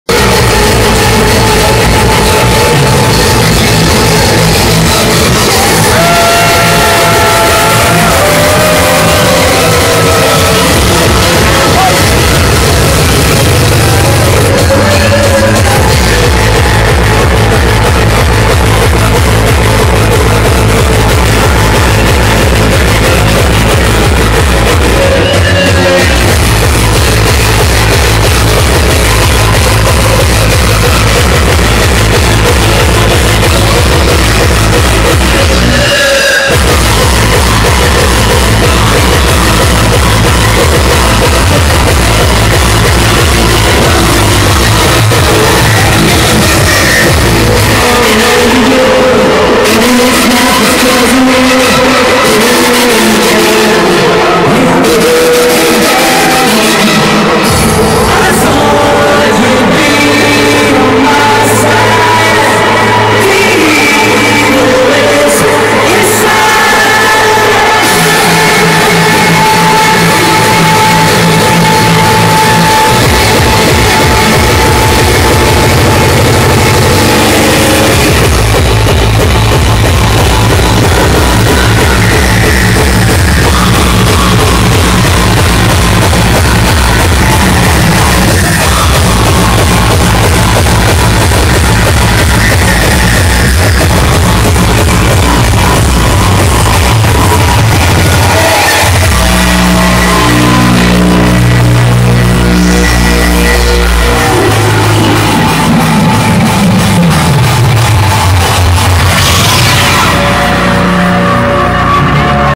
Paul Elstak (1) @ PONT AERI.MP4
Paul Elstak @ Paul Elstak Birthday

22/01/2011

Pont Aeri (Vallgorguina, Spain)